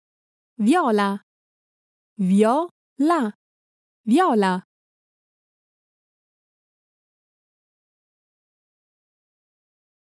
Viola
Viola (Viola in english) is a bowed string instrument. It is the middle voice of the violin family, between the violin and the cello.A person who plays the viola is called a violist or a viola player. The technique required for playing a viola has certain differences compared with that of a violin, partly because of its larger size: the notes are spread out farther along the fingerboard and often require different fingerings. The viola's less responsive strings and the heavier bow warrant a somewhat different bowing technique, and a violist has to lean more intensely on the strings.